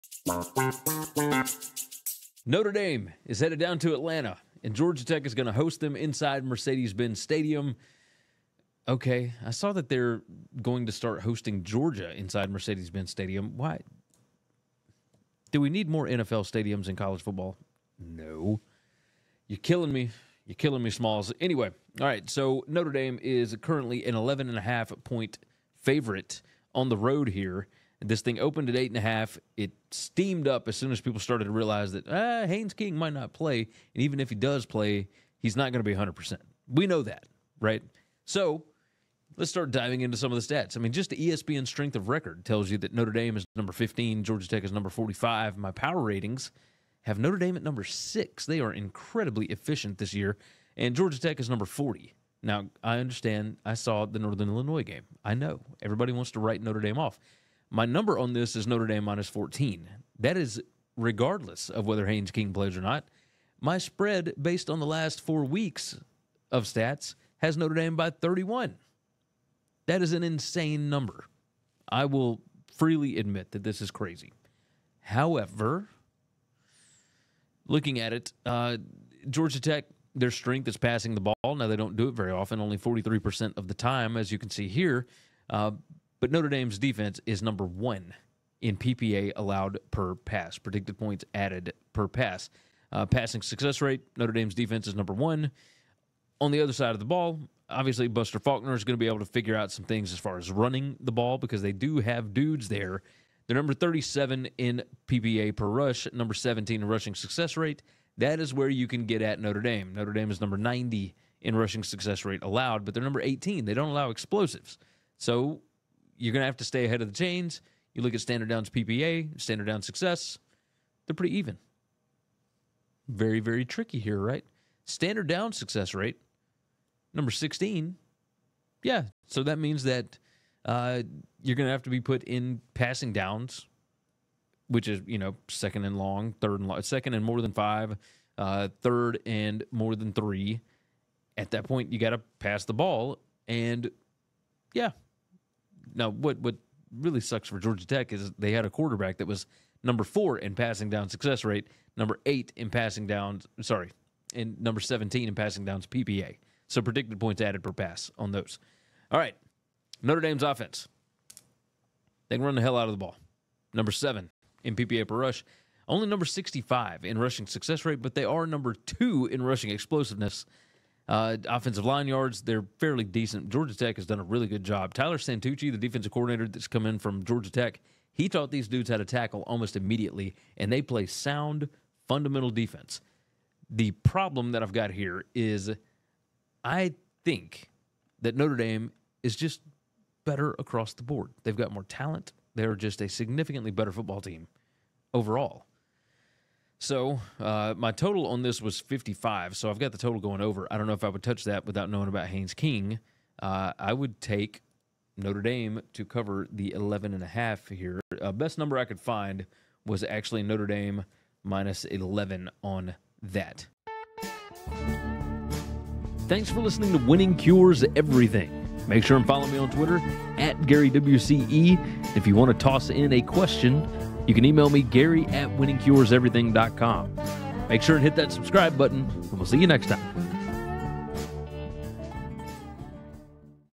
Notre Dame vs Georgia Tech Picks & Prediction Against the Spread 2024 College Football Analysis
Notre Dame vs Georgia Tech Picks Against the Spread 2024 Predictions

Gary gives a quick preview, analysis, score prediction based on advaWashingtoned stats and data & his pick (lean) against the spread (ATS) for the 2024 College Football Week 8 matchup featuring Georgia Tech vs Notre Dame!